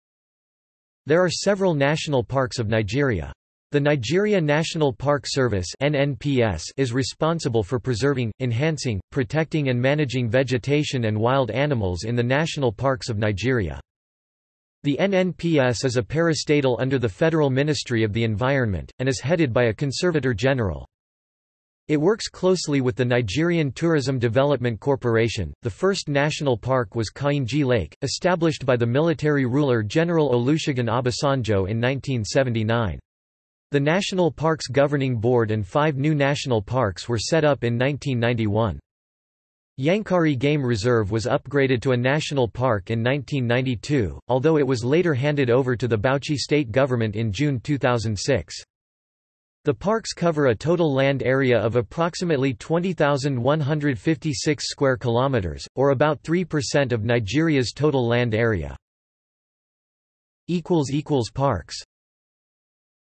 List of national parks of Nigeria | Wikipedia audio article
- Socrates

SUMMARY
There are several national parks of Nigeria. The Nigeria National Park Service (NNPS) is responsible for preserving, enhancing, protecting and managing vegetation and wild animals in the national parks of Nigeria.
The NNPS is a parastatal under the Federal Ministry of the Environment, and is headed by a Conservator General.
It works closely with the Nigerian Tourism Development Corporation.The first national park was Kainji Lake, established by the military ruler General Olusegun Obasanjo in 1979. The National Parks Governing Board and five new National Parks were set up in 1991.
Yankari Game Reserve was upgraded to a national park in 1992, although it was later handed over to the Bauchi State government in June 2006.
The parks cover a total land area of approximately 20,156 km2, or about 3% of Nigeria's total land area.